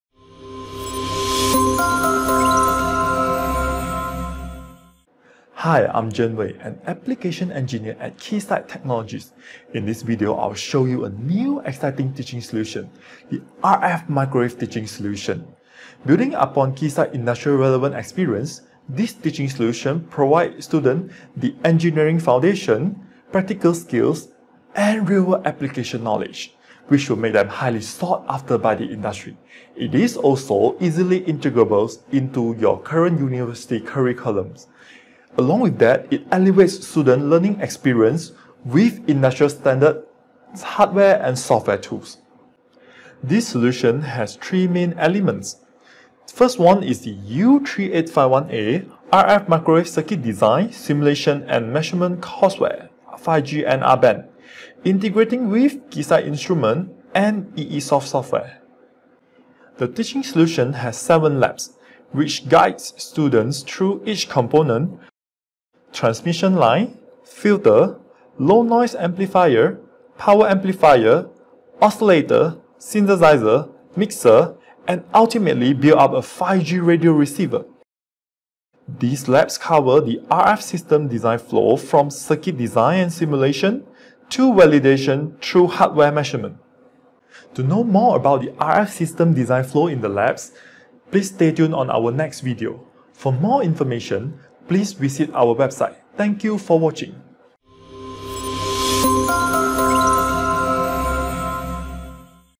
Keysight RF Microwave Teaching Solution introduction and overview
To prepare industry-ready students, Keysight’s RF Microwave Teaching Solution focuses on the complete RF circuit design flow, from design specifications and simulation to prototype building and validation, operating on the 5G New Radio Band n3 downlink frequency. This gives students a solid foundation in RF microwave fundamentals and paves the way for them to specialize in more advanced wireless applications in areas such as 5G and IoT.